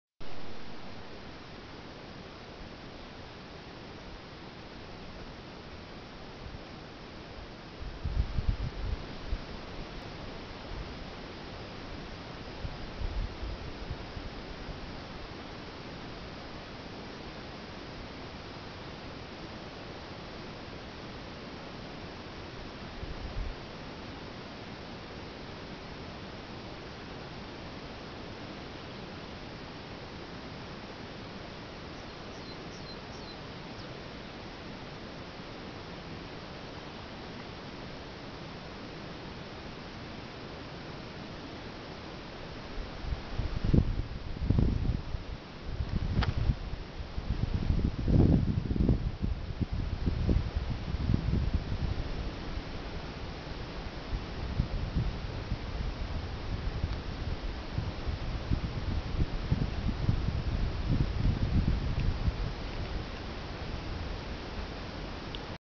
Sentiero per monte Palanzone
Ripreso dal sentiero che porta al monte Palanzone dalla colma di Sormano.